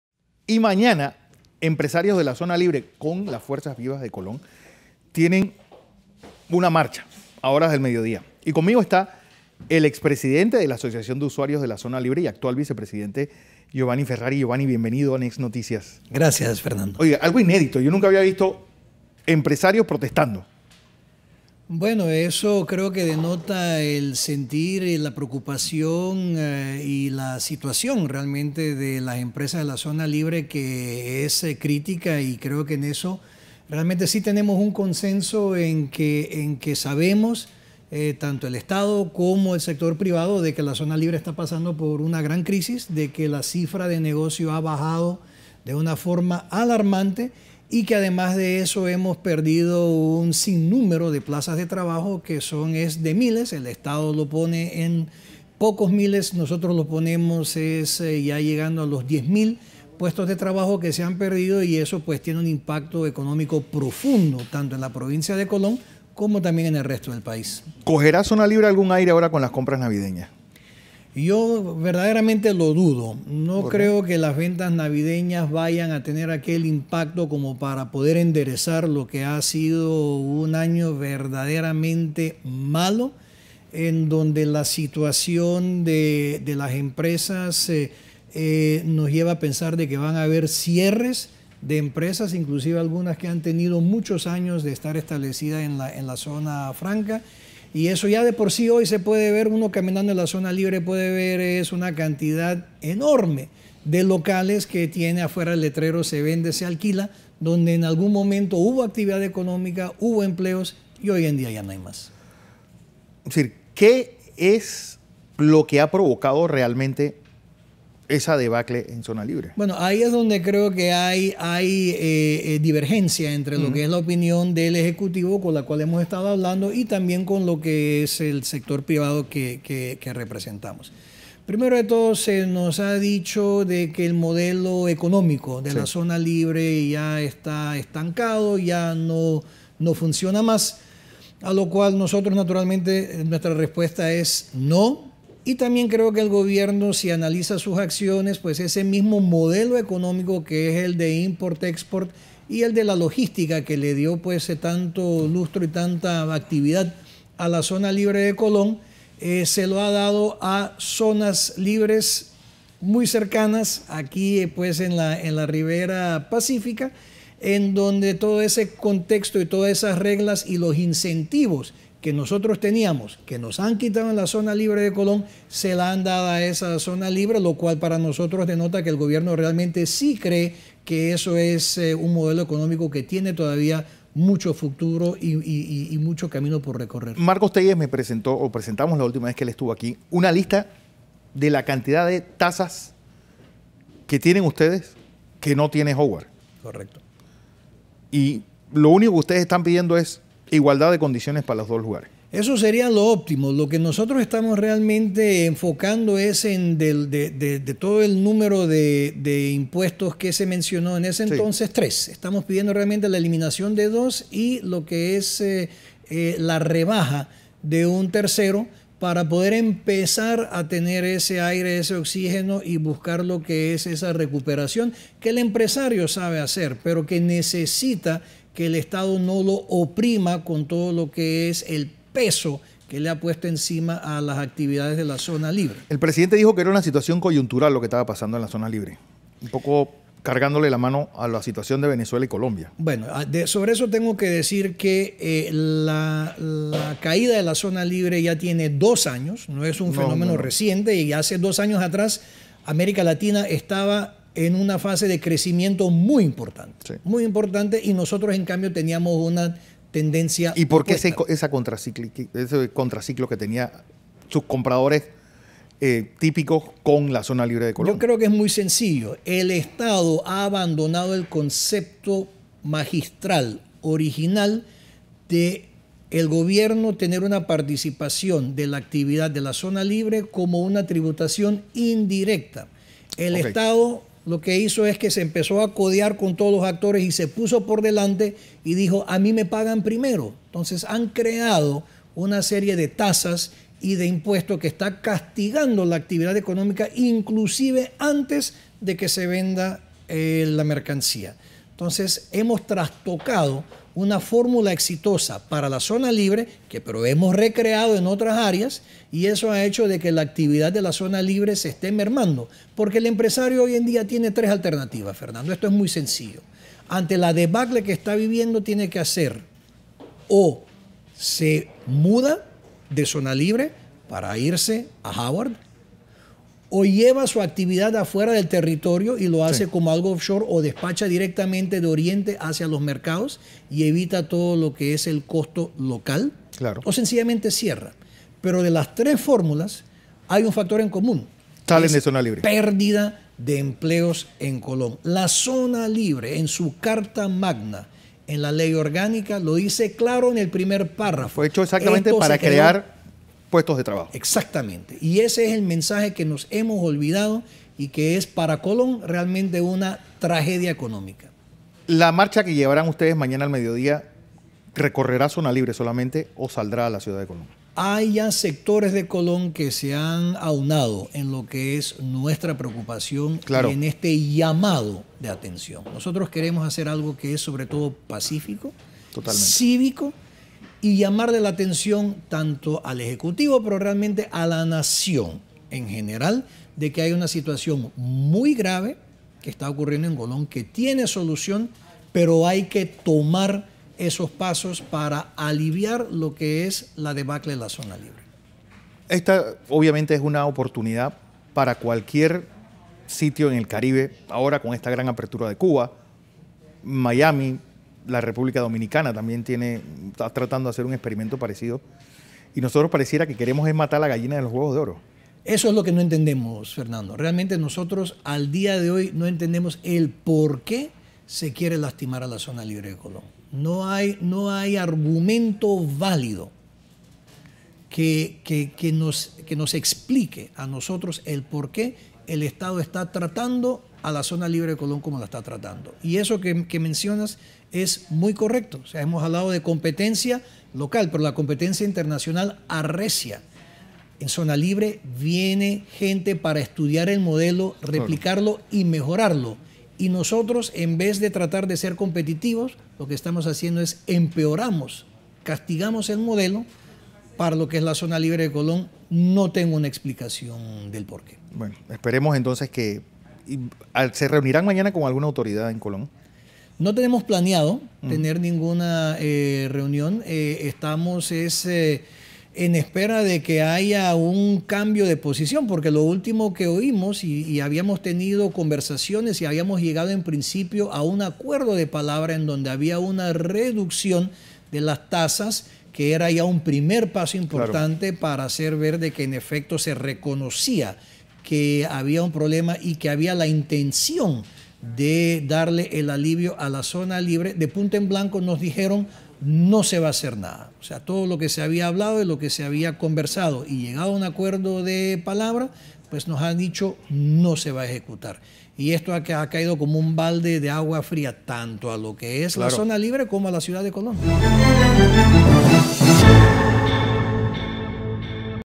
Empresarios y usuarios preocupados situación en la zona libre de Colón
Giovanni Ferreri de la Asociación de Usuarios de la Zona Libre de Colón manifestó este miércoles en nuestro noticiero matutino la preocupación que existe entre empresarios y usuarios por la gran crisis que atraviesa la Zona Libre de Colón. Se habla de una ola de perdida de 10 plazas de trabajos y decenas de cierre de empresas.